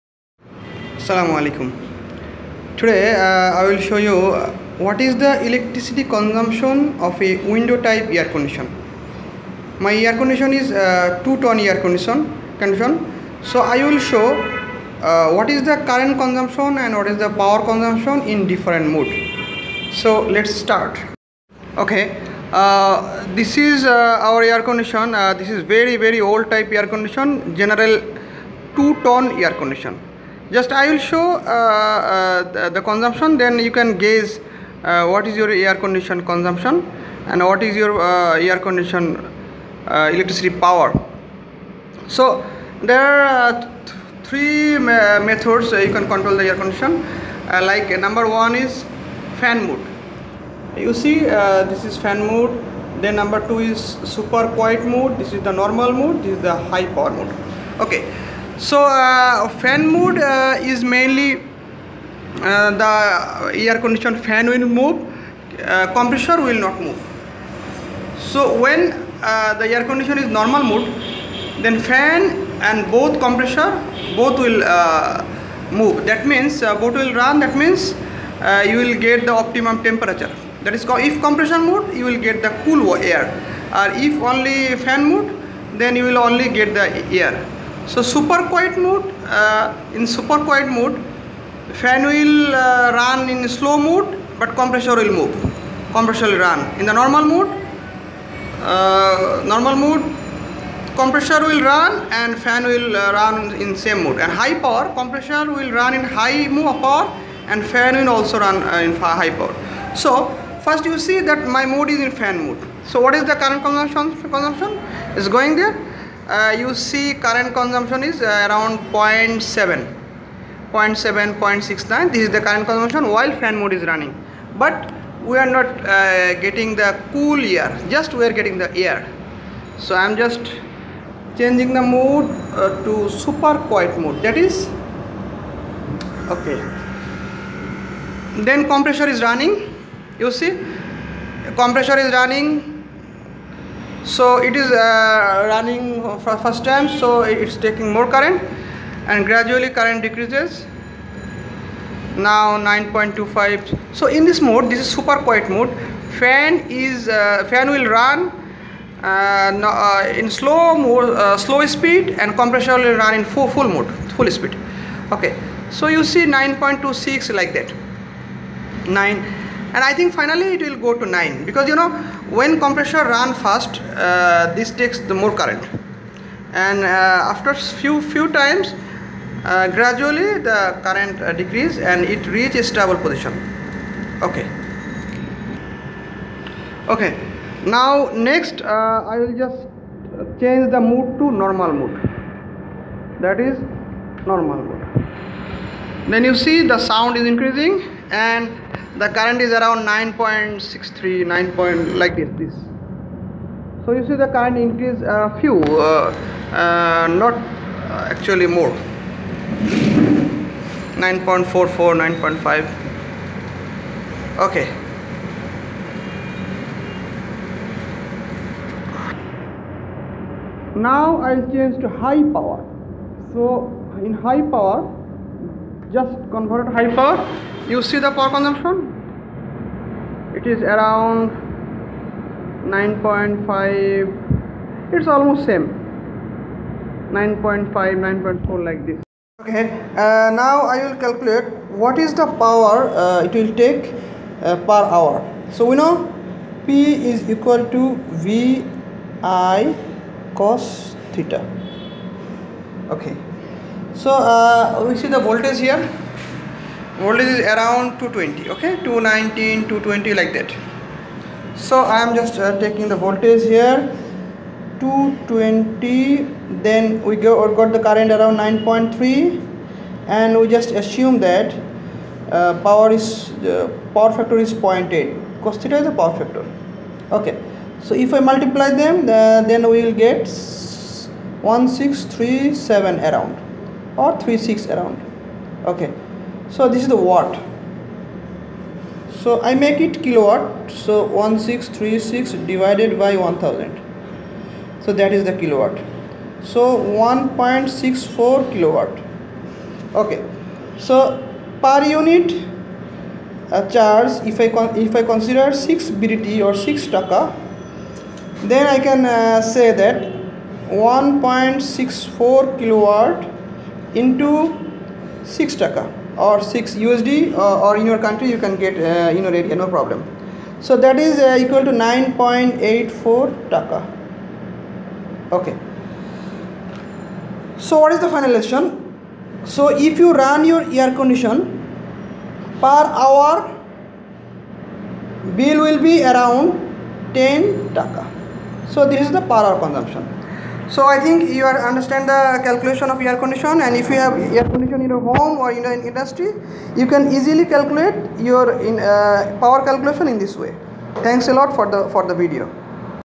Power Consumption of Window Type Air Conditioners
Do you know what's the current and power consumption in your window type AC.
Per hour bill for running your AC
Watt consumption for your Windows Type General AC
2 Ton